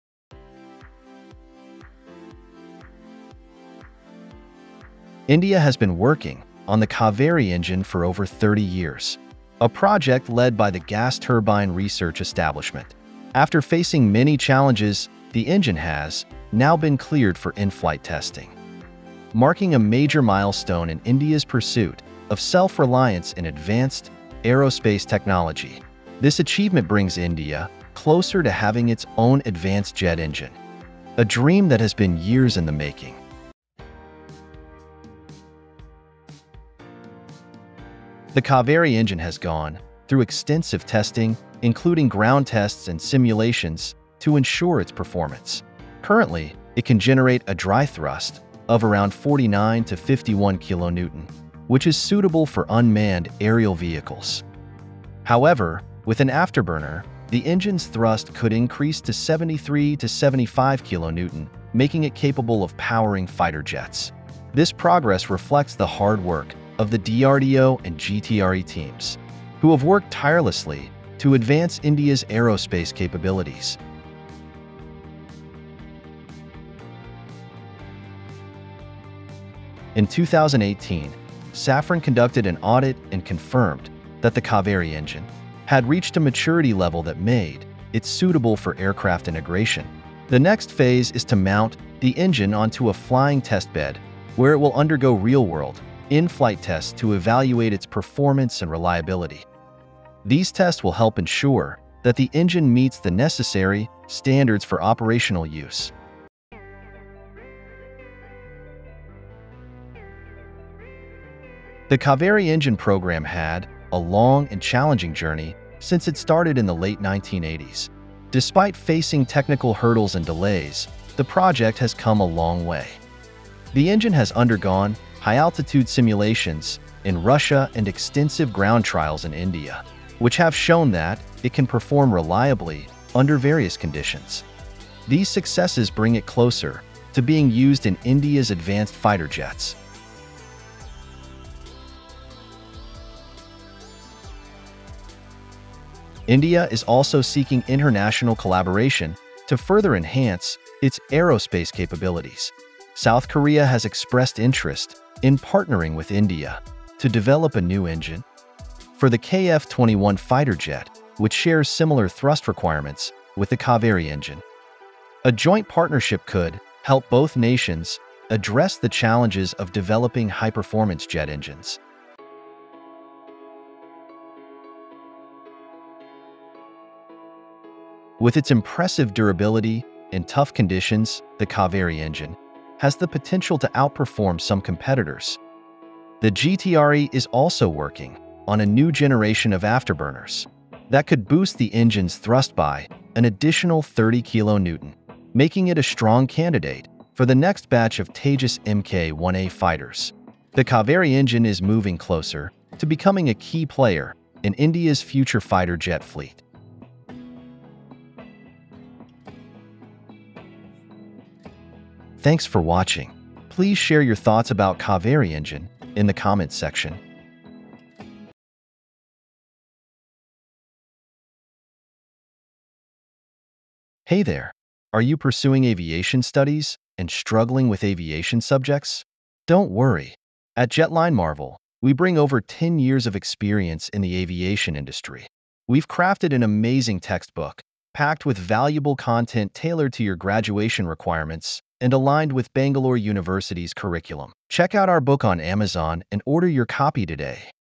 Is the Kaveri engine ready for testing trials, India's much-anticipated fighter jet engine?
India has been working on the Kaveri engine for over 30 years, a project led by the Gas Turbine Research Establishment (GTRE). After facing many challenges, the engine has now been cleared for inflight testing, marking a major milestone in India's pursuit of self-reliance in advanced aerospace technology. This achievement brings India closer to having its own advanced jet engine, a dream that has been years in the making.

The Kaveri engine has gone through extensive testing, including ground tests and simulations, to ensure its performance. Currently, it can generate a dry thrust of around 49-51 kN, which is suitable for unmanned aerial vehicles (UAVs). However, with an afterburner, the engine's thrust could increase to 73-75 kN, making it capable of powering fighter jets. This progress reflects the hard work of the DRDO and GTRE teams, who have worked tirelessly to advance India's aerospace capabilities.